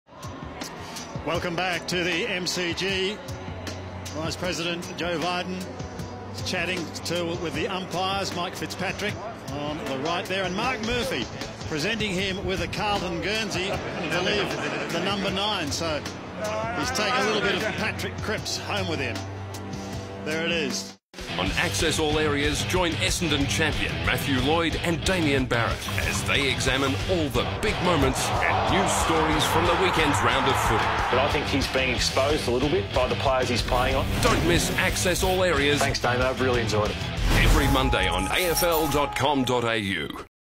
The VP at the 'G - AFL
US Vice President Joe Biden is presented with a Carlton jumper before the game at the MCG.